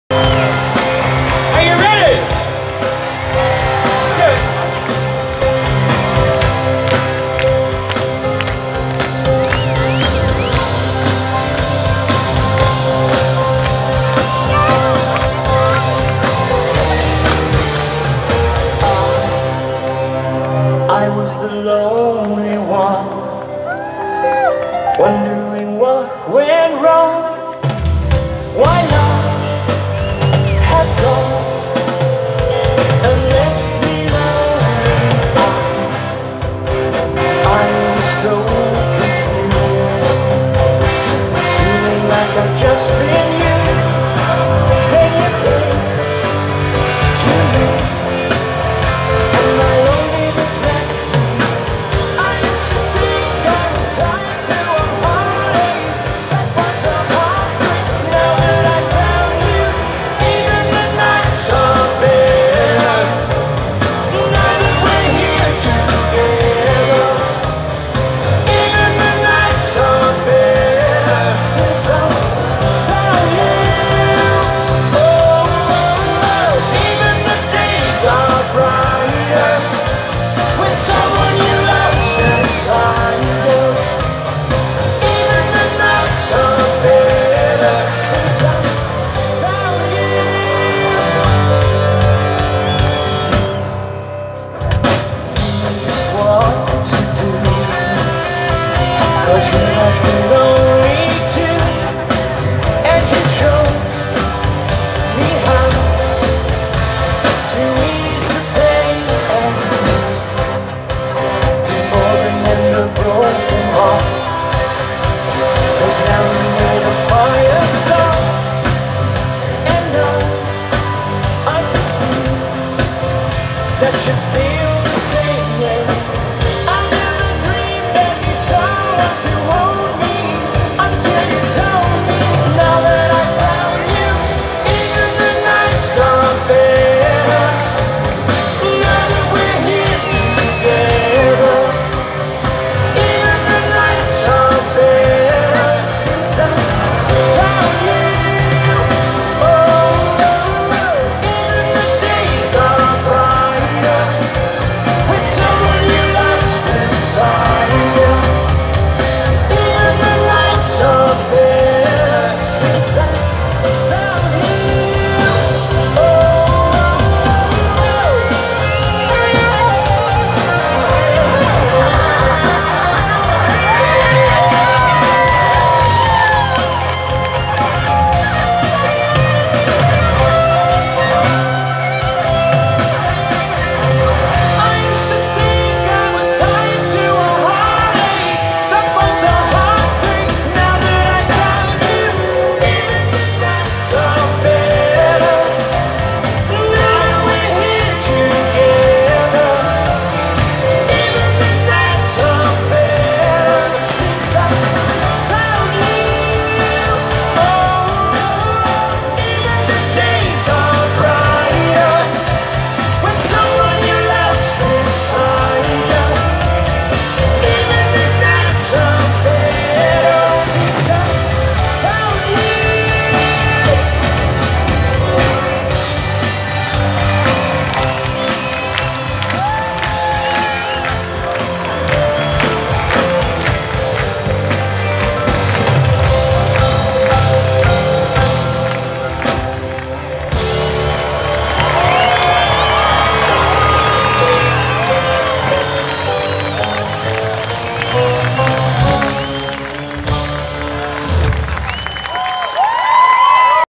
En concierto AIR SUPPLY en La Paz
El 03 de Noviembre se presentó AIR SUPPLY en el Estadio Arturo C. Nahl de La Paz